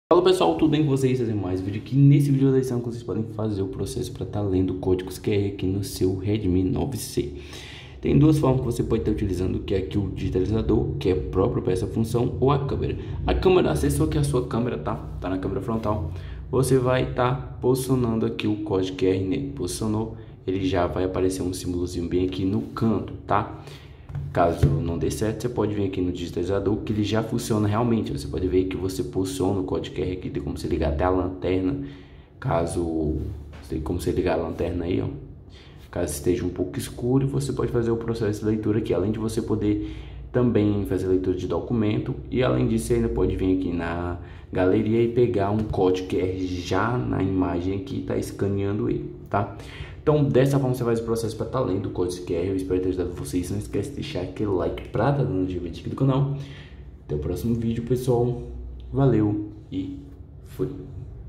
Como ler códigos Qr no seu Redmi 9C - passo a passo
Fala pessoal!! vou mostrar nesse video passo a passo de como ler códigos Qr no seu Redmi 9C

✩ Redmi Note 9 64G / 128GB
✩ Redmi Note 9S 64G / 128GB
✩ Poco X3